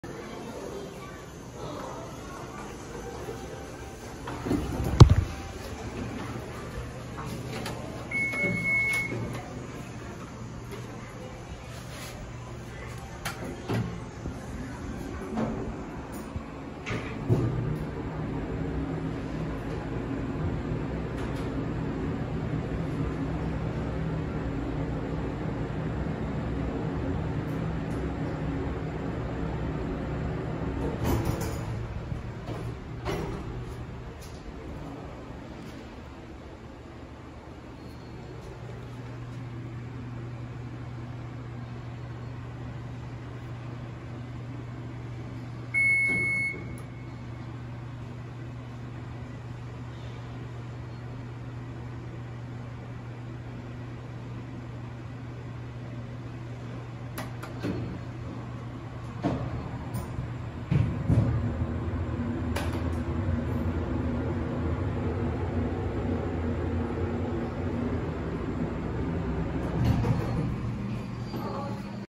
山陽マルナカ 笠岡店のエレベーター
このエレベーターは、シンドラーで稼働しています。無操作のみ戸閉ブザーが鳴ります。
人が多かったので途中の２階で打ち切りしました。インジケーター部分とランプが分離していますが、数字部分は、連動しています。

メーカー：シンドラー
アナウンス：なし
到着チャイム：なし
停止可能階：１，２，３

その他
戸閉ブザー：あり